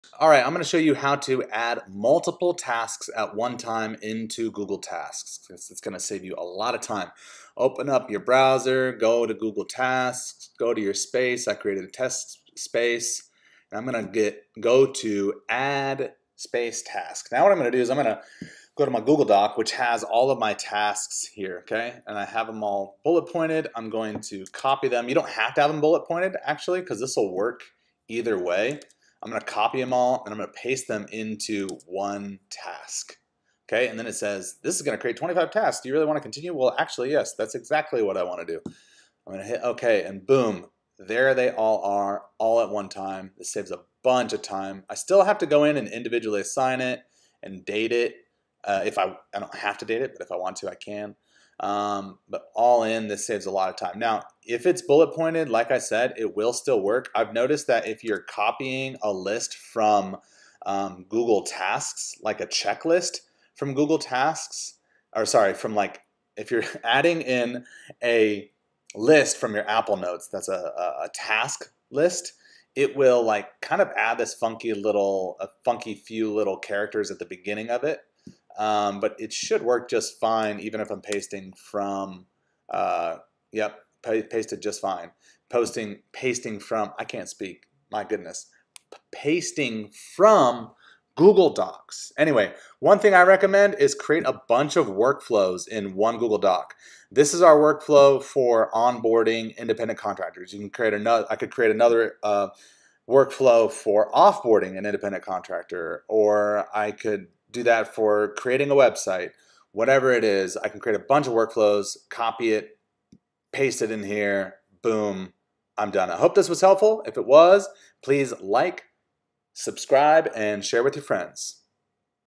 How to Add Multiple Tasks at One Time to Google Tasks
In this video, I show how to very easily add multiple tasks at one time to Google Tasks.